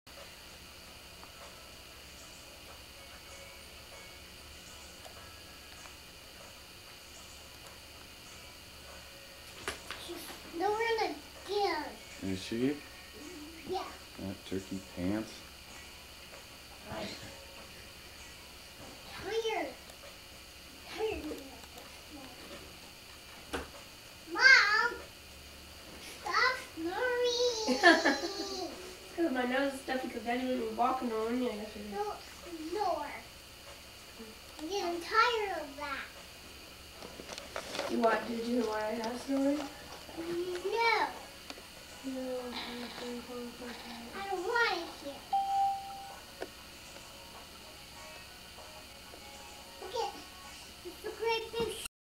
Philly Kelsi play Comp Sat mom stop snoring again 51 s Clip DONE.mpg
Kelsi Colton playing the computer in Philly before her treatment for her cancer.